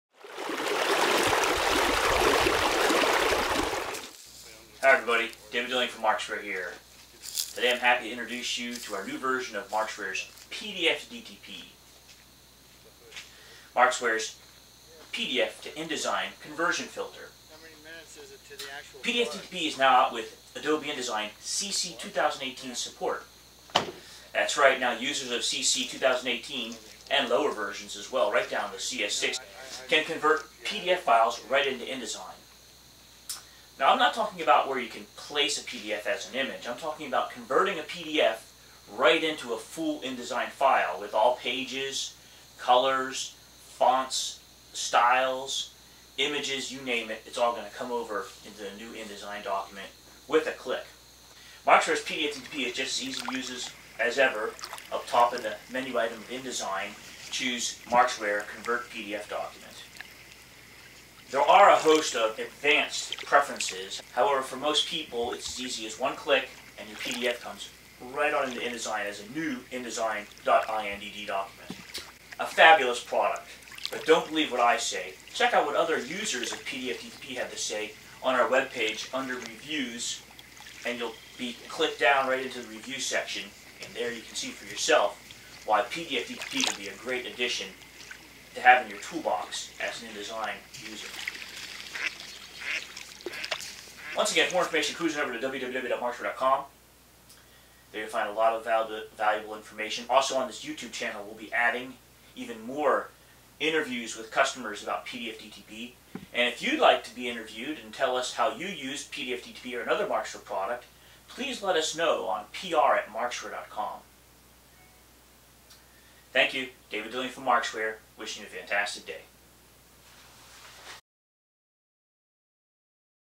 InDesign PDF Import - CC 2018 support for PDF2DTP from Markzware
InDesign PDF import, with CC 2018 support for PDF2DTP from Markzware: PDF to InDesign document conversion is a way to import full PDF layout content! PDF2DTP with support for Adobe InDesign CC 2018 (Creative Cloud’s latest version, at the time of this video's creation) support allows you to open PDF files within InDesign on Macintosh Apple.

Import or convert all pages, styles, fonts, images, colors, and more! Markzware's PDF converter is easy to use and comes with advanced and professional preferences for graphic design, publishing or prepress operators who want more. It even gets tables and the likes into InDesign.

Why edit a PDF with a PDF-editor? Work within the app that you're used to, Adobe InDesign. It does not matter where the PDF came from, Markzware's solution will convert them, for you, and even extract all images, at effective resolution used!

Note: Markzware's stand-alone PDFMarkz application replaces the PDF2DTP InDesign plug-in and allows users to open PDF files in InDesign, Affinity Publisher, and other formats